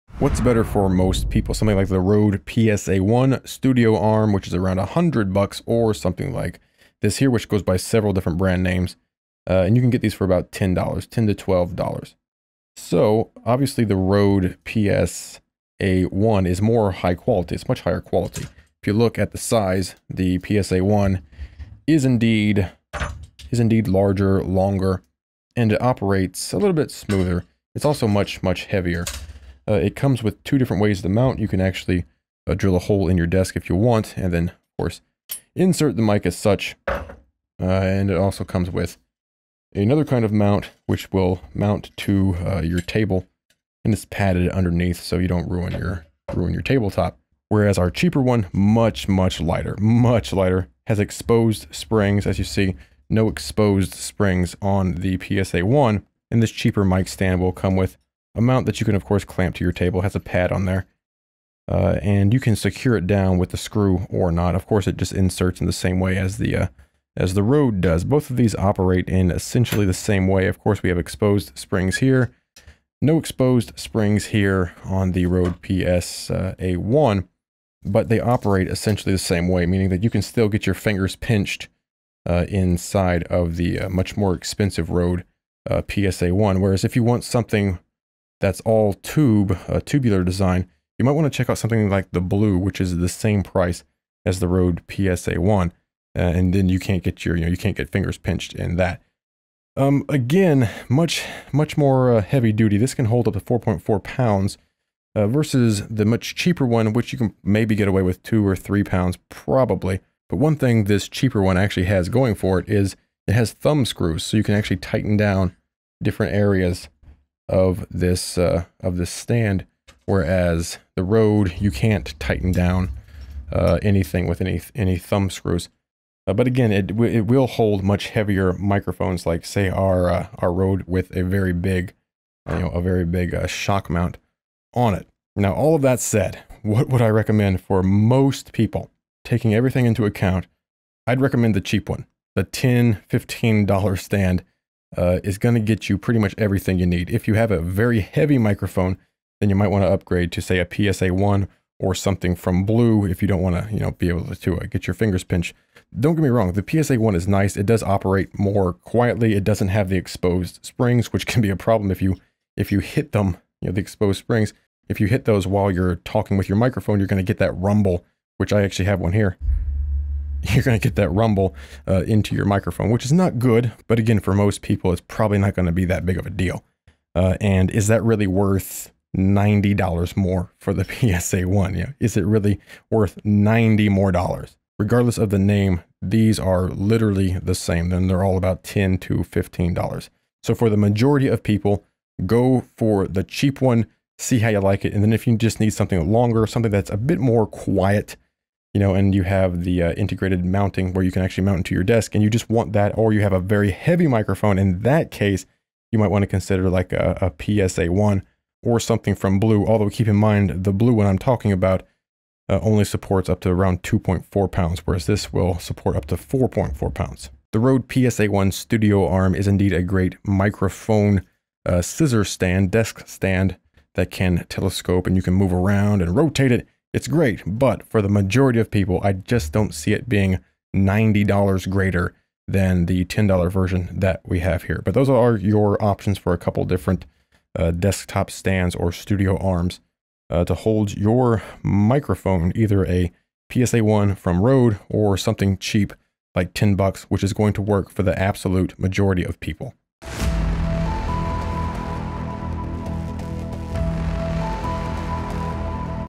$10 vs $100 Microphone Studio Boom Arm 🔥
The Rode PSA1 is a nice mic arm that includes two mounting options, quiet operation, and holds a lot of weight. It costs $100.

The cheap mic arm has exposed springs, it doesn't operate quiet as smoothly, but overall it's not bad for only around $10.

Both of these are good options, and really, I'd suggest getting a couple cheap arms just to have around even if you go with the Rode PSA1. If you have a podcast it's a good idea to have multiple arms ready to go just in case.

Cheap Mic Boom Scissor Arm

Ethereum:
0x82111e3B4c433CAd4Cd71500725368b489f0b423

Bitcoin:
17ZDtMBXopFK4tpwG3iXU9WFznG65CgCJD

Bitcoin Cash:
127HvctzcxvGwP8LAamG7K8XSqPV4JzU2G

Litecoin:
LMdsQxvwo72aboVB2fA5kiq9YwyFNkvMKY